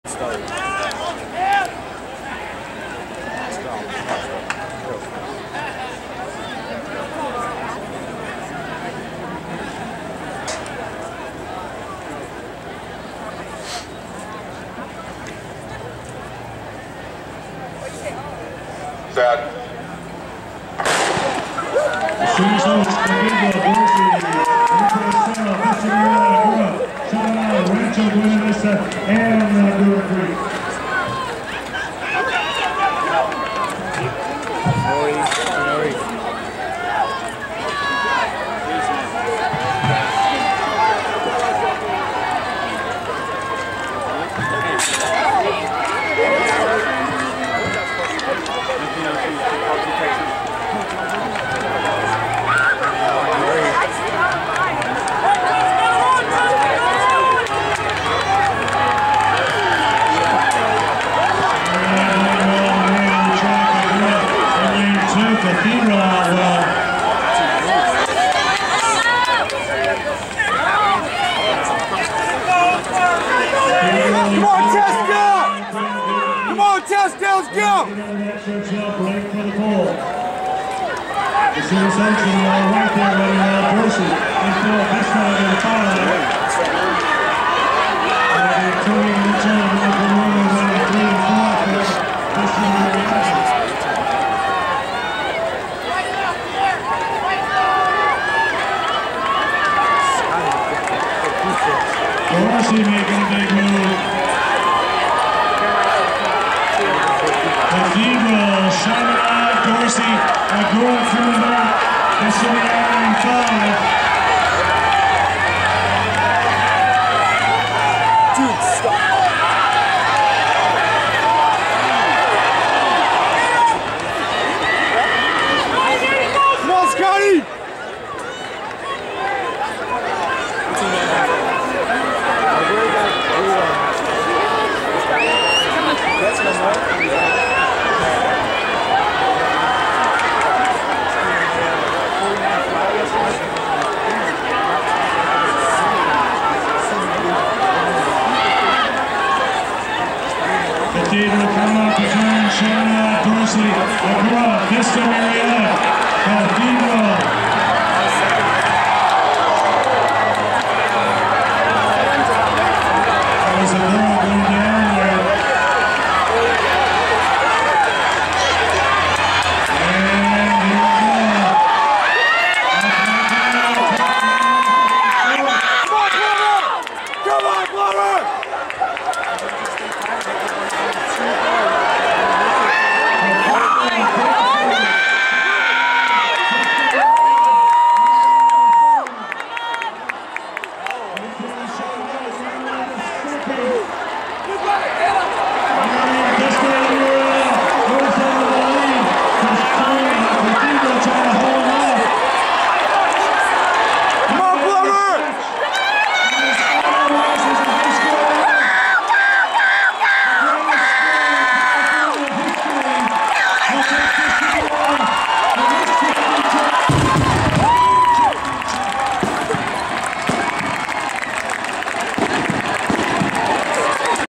2016 CIF State Finals 4x400m Relay Team
6th Place - Noriega Moffett, Jeremiah Testa, Scott Fitzpatrick, Armon Plummer